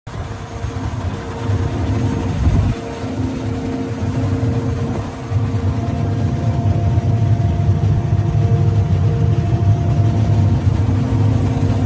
2014 JOHN DEERE S680 For Sale
2014 JOHN DEERE S680 with ALOT of Options! HID lights, Leather Seat, Serial # 1H0S680SLE0767205, 2760 Engine Hours/ 2073 Separating Hours, 2WD, and a ProDrive Transmission w/Harvest Smart Feedrate Control! Premium Radio with XM, Premium Cab, Heavy Duty tilt cyclinder, Contour Master (Lateral Tilt Feederhouse), Sidehill Perforance Package, Chopper, Corn/Bean, 26' PowerFold Unloading Auger, and a Extended Wear Round Bar Concaves! Feed Accelerator Slow Speed DR, GreenStar 3 2630 Display, Additional "Full" Grain Tank Sensor, and MUCH MORE! Located in Velva, ND.